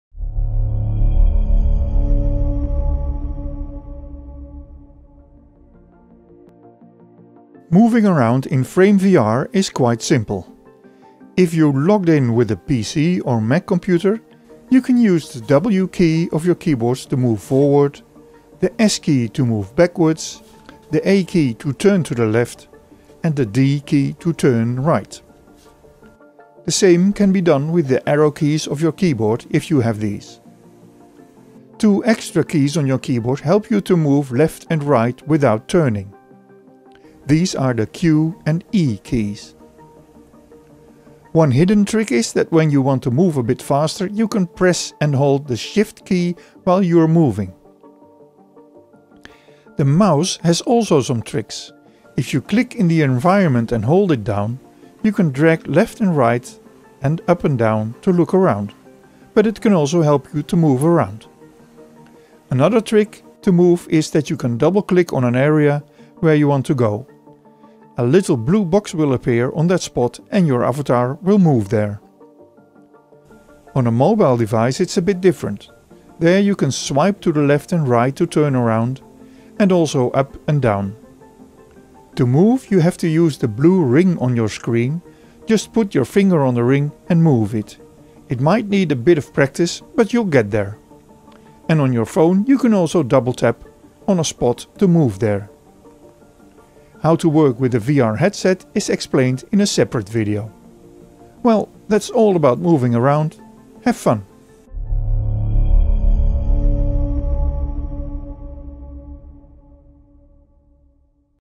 Moving around in FrameVR
This video explains how to move around on a PC or MAC and on a mobile phone, when you are in a FrameVR environment.
NOTE: With a QWERTY keyboard you have to use the WASD keys to move. If you have a AZERTY keyboard, use the ZQSD keys.
More information about education in FrameVR can be found on our